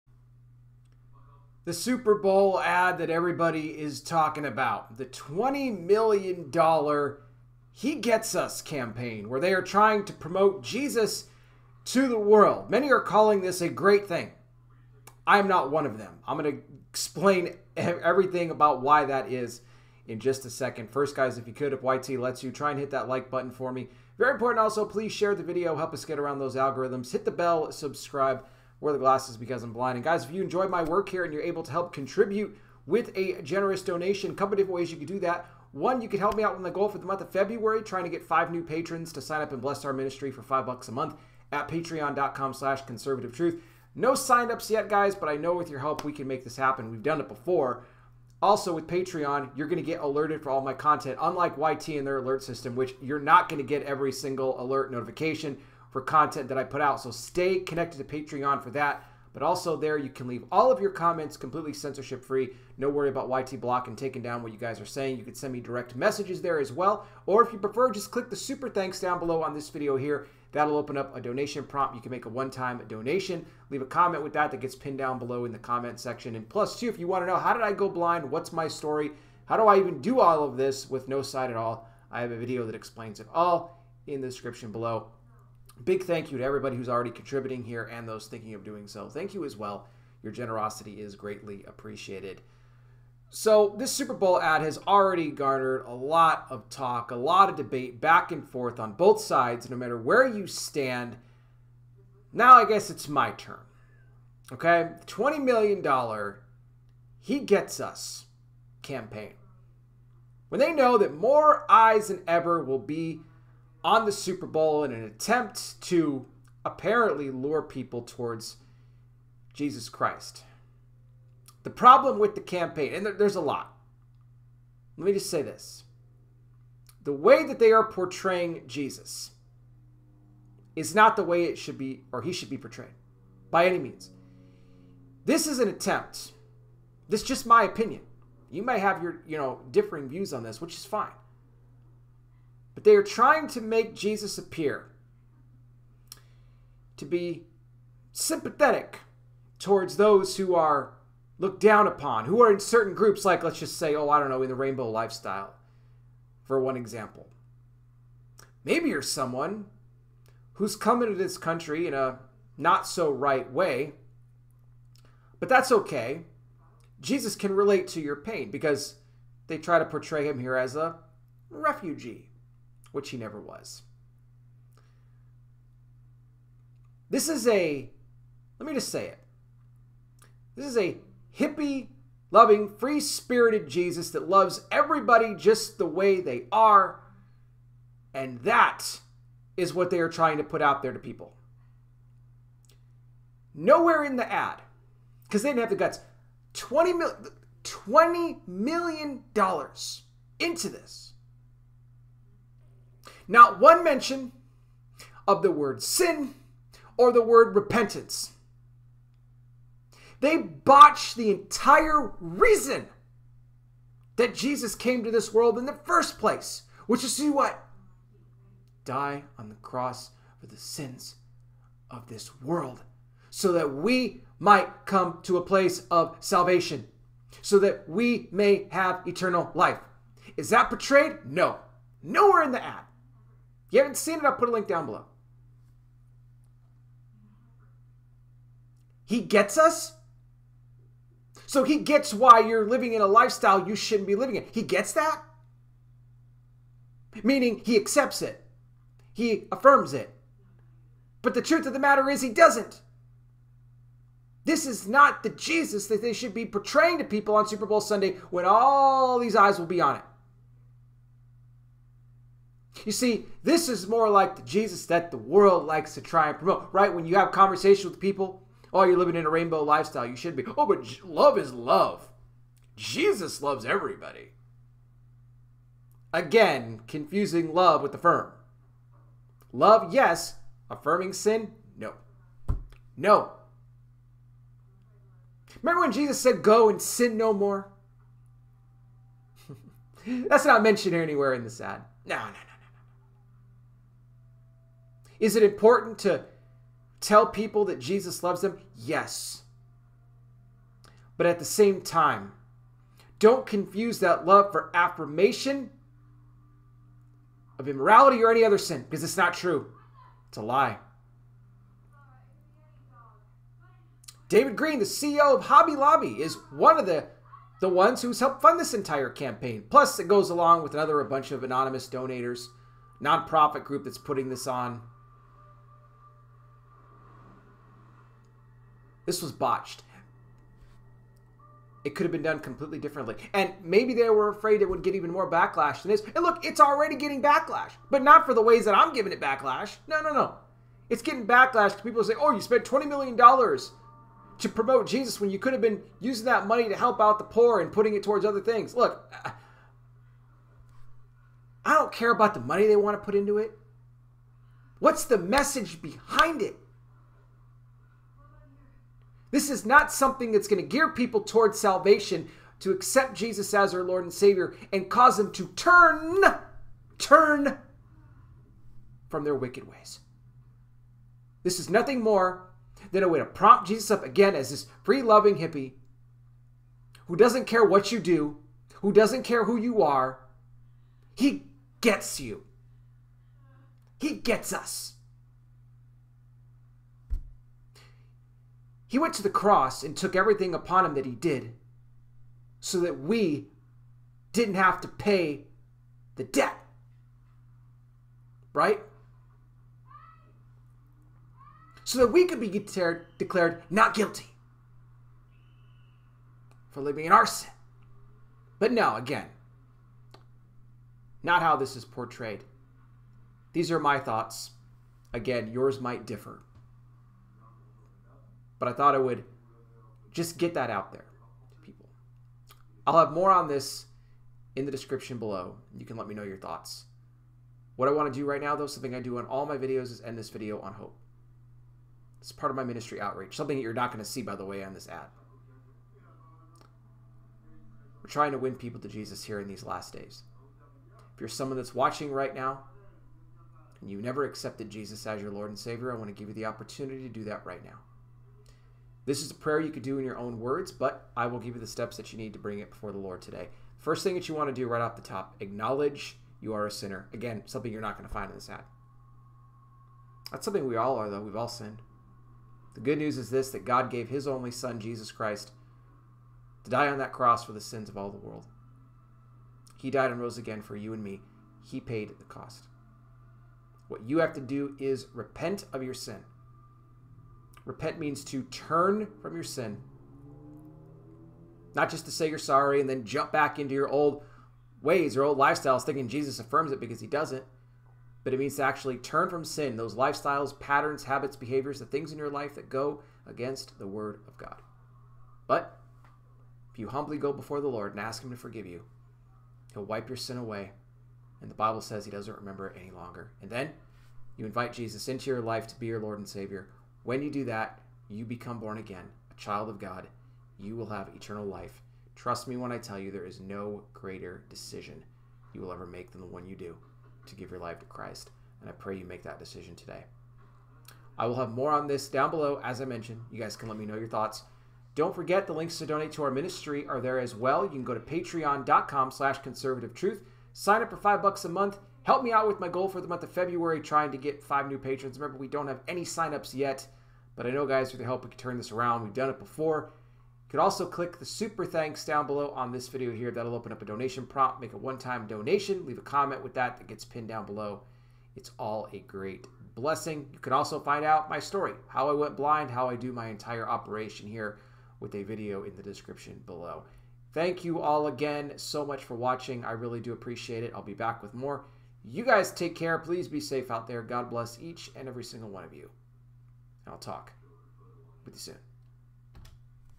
Super Bowl $20M 'He Gets Us' Jesus Campaign Is A WOKE JOKE Rpt 11th Feb, 2023
MY STORY ON GOING BLIND & HOW I OPERATE MY VIDEO MINISTRY